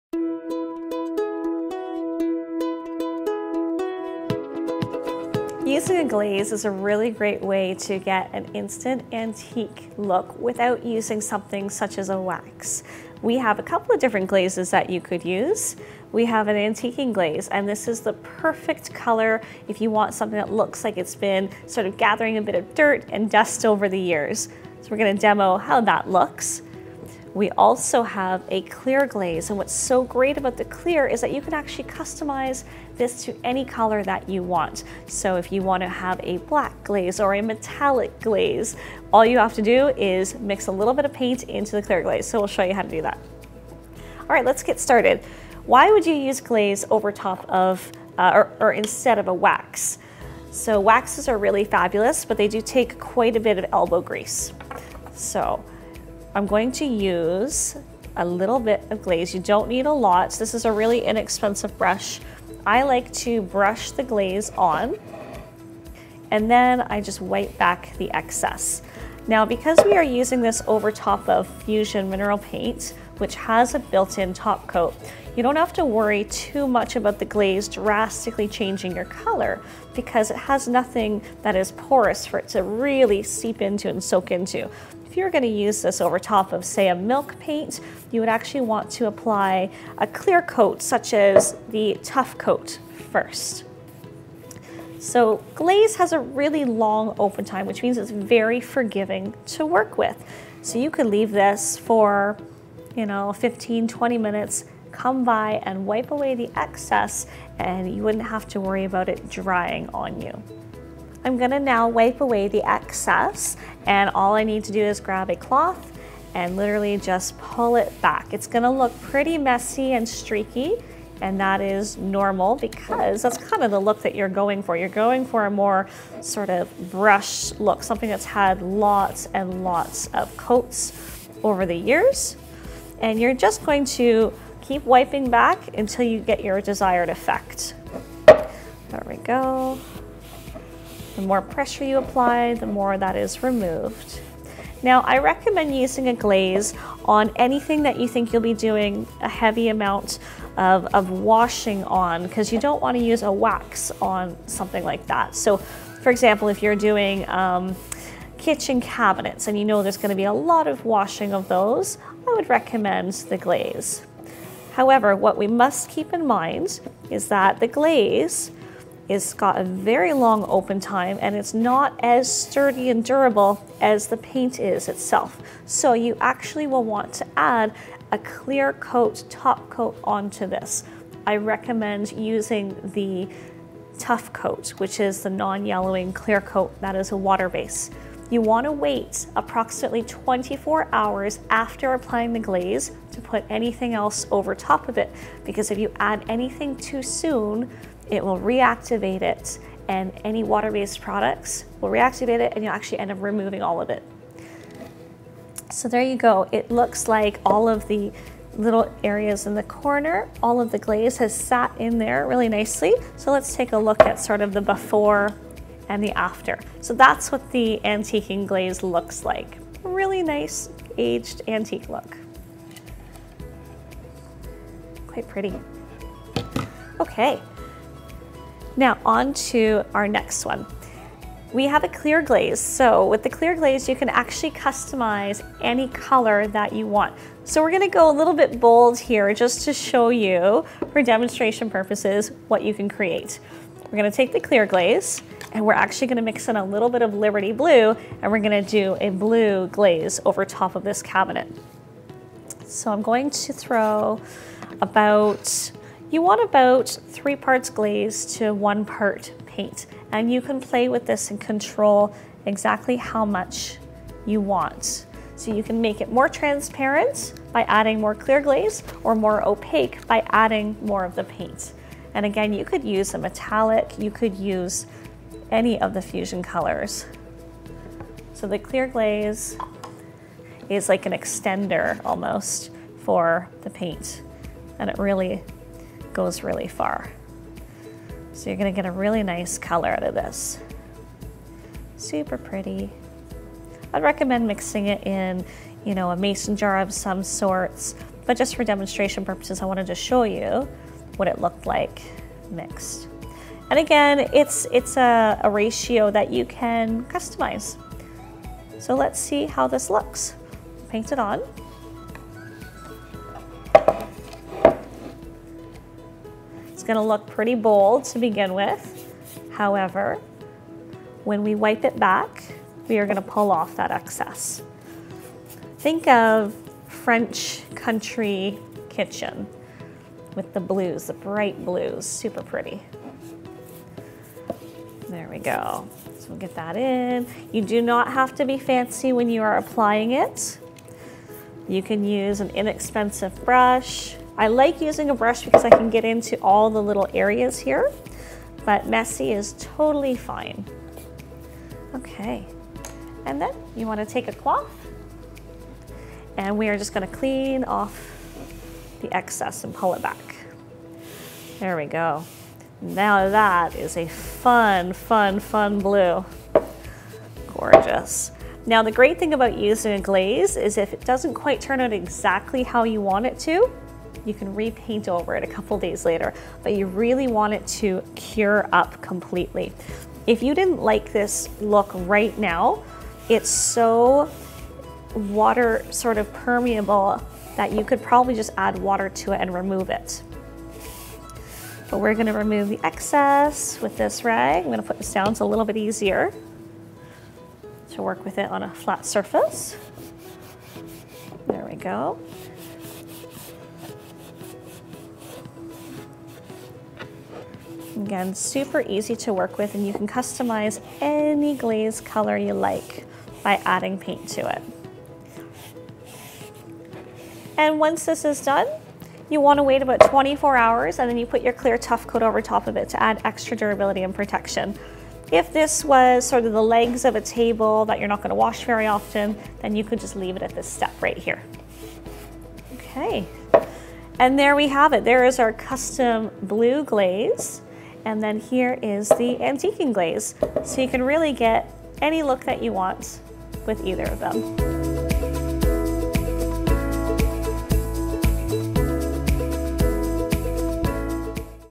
How To Use Glaze | Fusion™ Mineral Paint
Sometimes all you need is quick a coat of glaze to give your favourite furniture pieces an antique, vintage-esque touch... Watch the magic happen as we use demonstrate how each of glazes work!

(P.S. The Clear Glaze and a splash of your favourite Fusion paint hue is a great way to achieve the perfect burst of colour for spring...)

Timecodes
0:00 Intro
0:07 Antique look with Glaze
0:44 Why Glaze over Wax
1:00 Applying Antiquing Glaze by Fusion
1:40 Wiping the Antiquing Glaze
3:50 How to customize Clear Glaze
5:08 Applying Clear Glaze by Fusion
7:13 Comparison